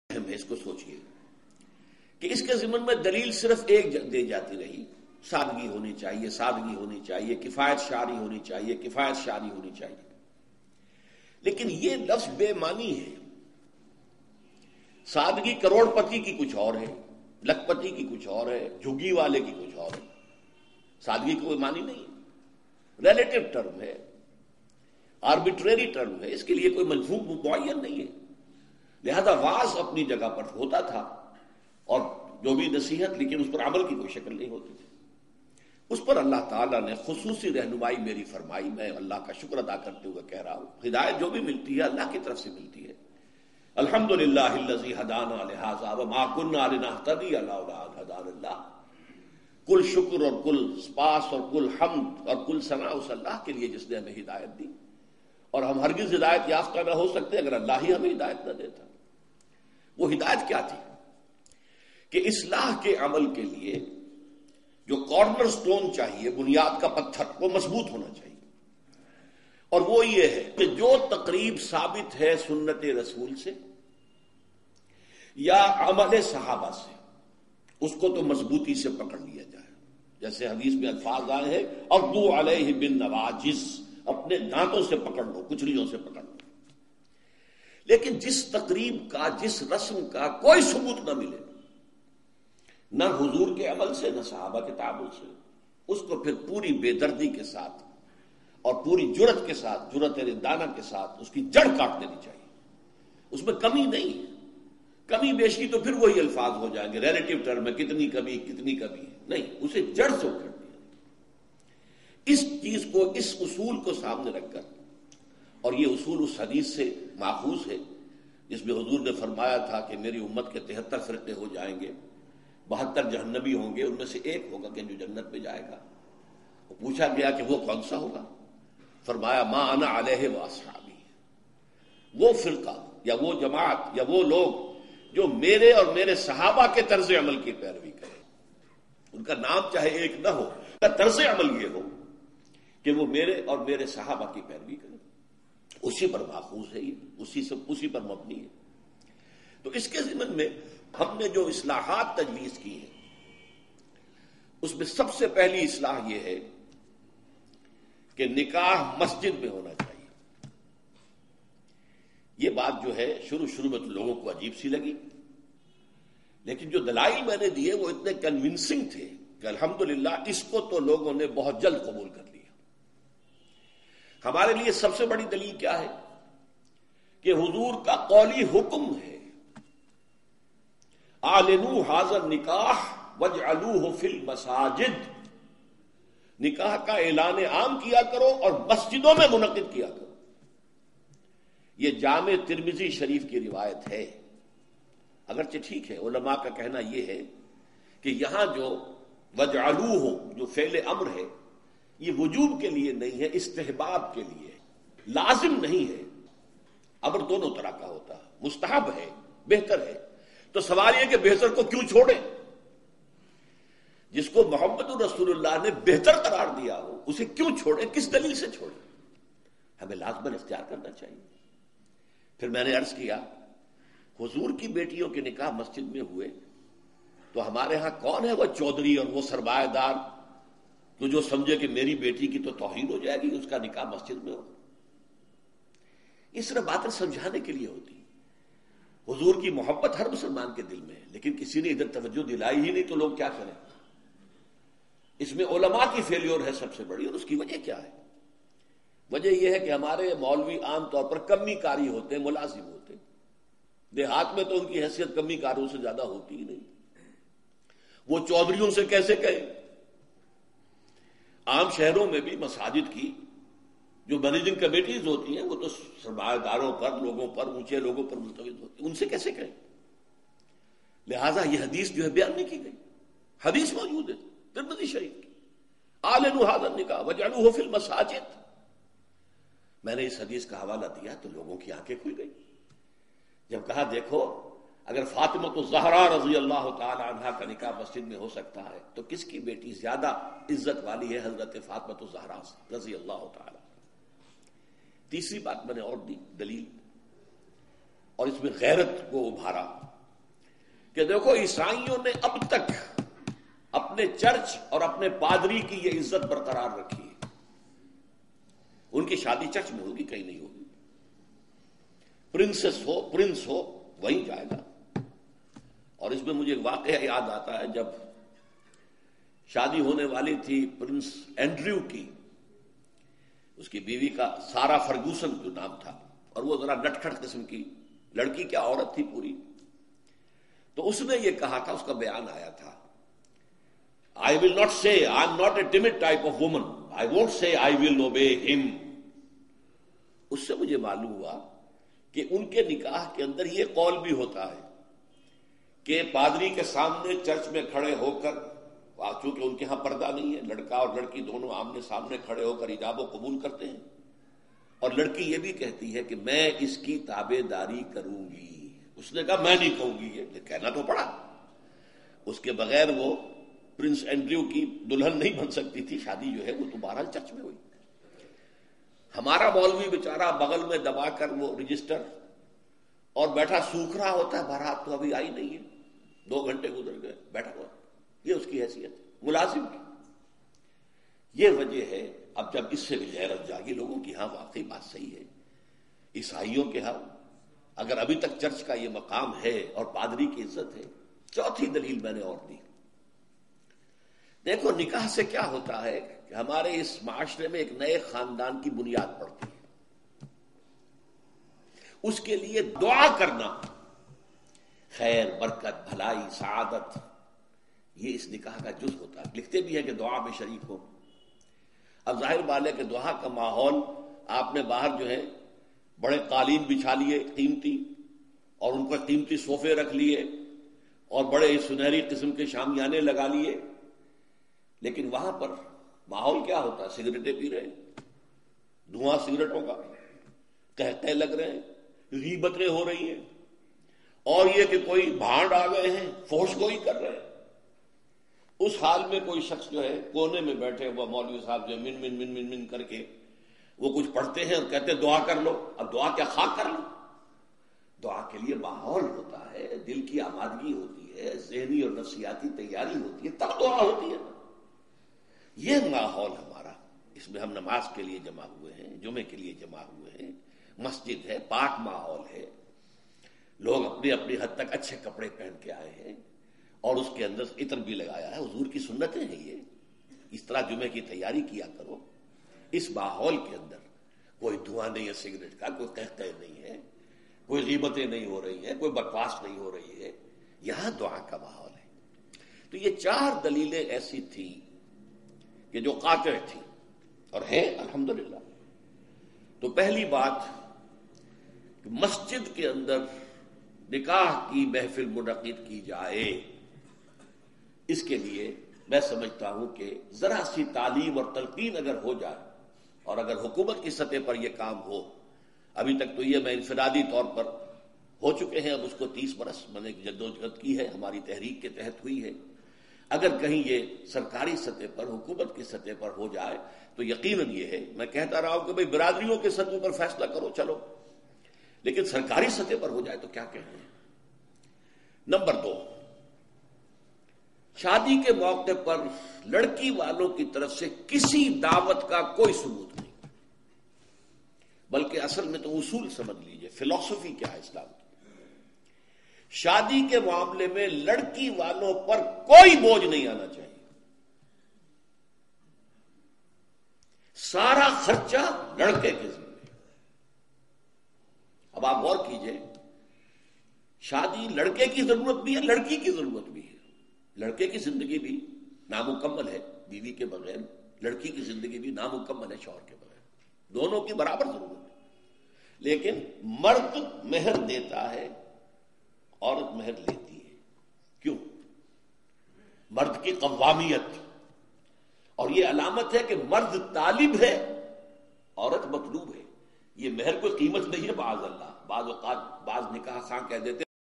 (2/6) Shaadi Ki Takribat Mein Islah (Dr. Israr Ahmed)
A public lecture by Dr. Israr Ahmed

Tanzeem-e-Islami Quran Academy Markazi Anjuman e Khuddam ul Qur'an Lahore IRF Peace TV Urdu Qtv Dr. Zakir Naik